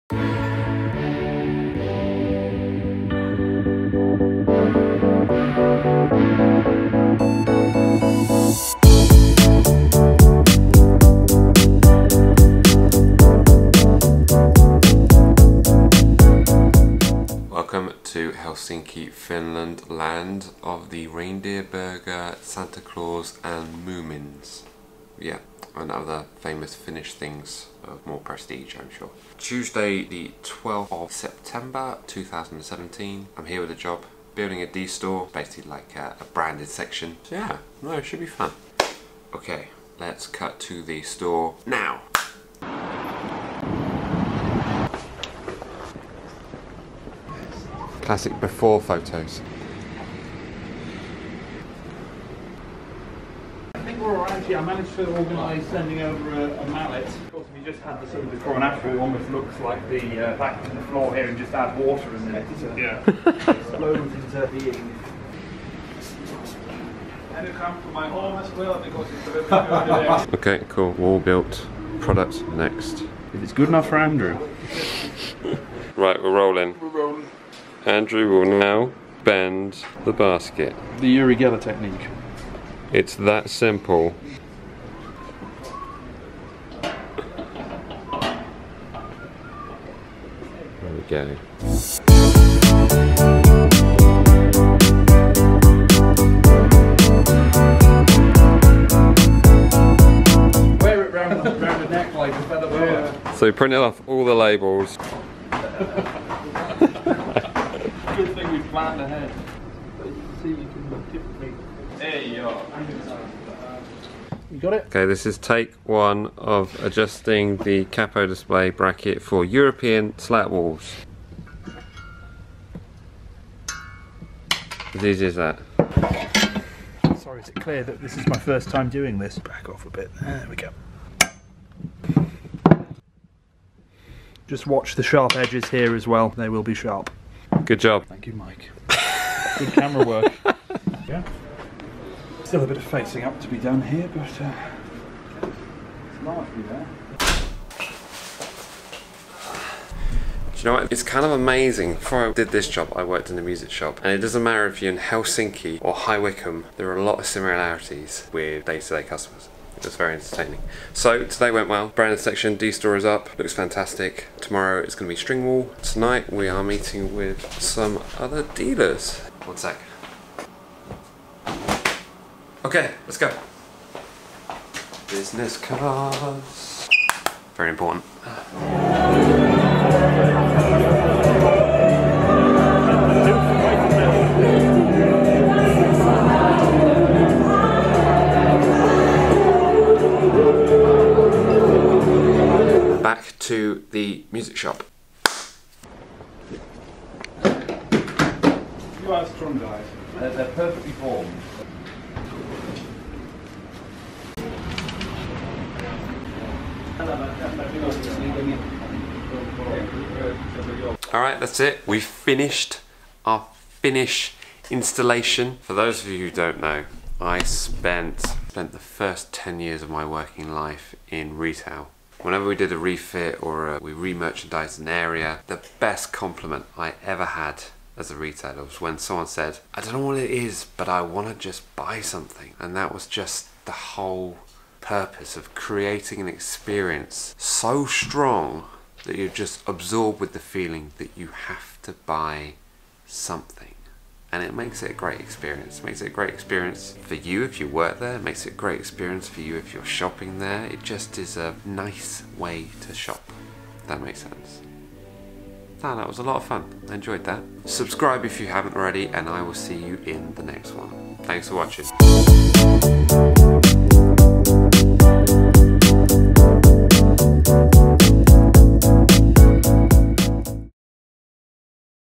Guitar String Heaven?! - Helsinki Finland 2017
Guitar String Heaven?! - Helsinki Finland 2017.  Out on the road for the company, working on in store merchandising